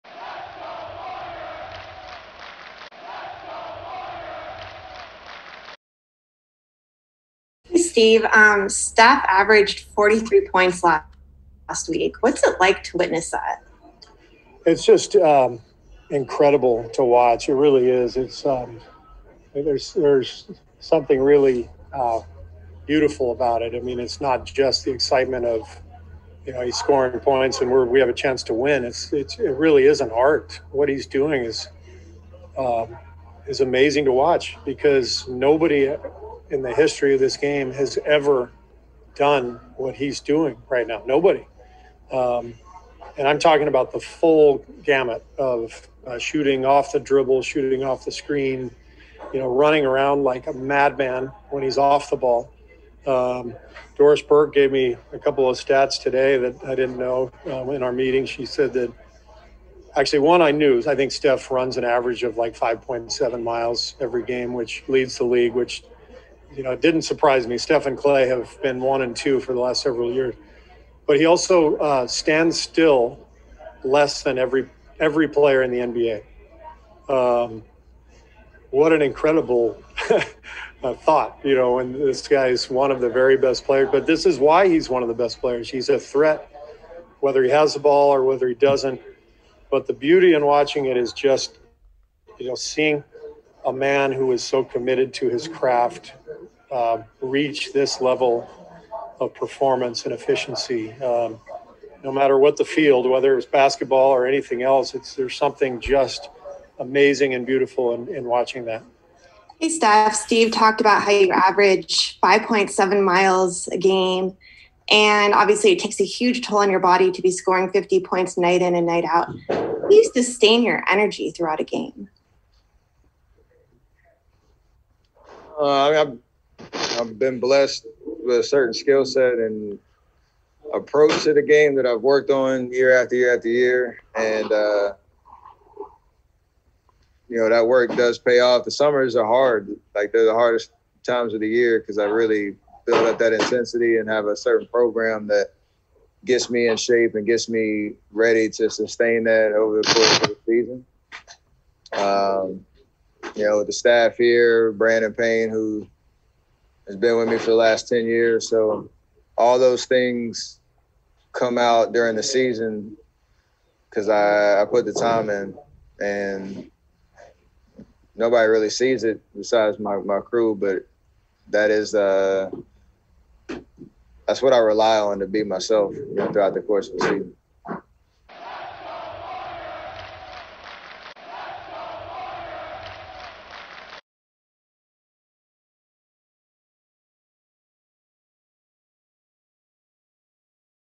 📺 Doris Burke stat on Stephen Curry: stands still less than every player in NBA; Brandon Payne
Steve Kerr and Stephen Curry interview from official Golden State Warriors (29-29) call at pregame and postgame after win at Sixers, held at Wells Fargo Center in Philadelphia, PA.

PODCAST DETAILS...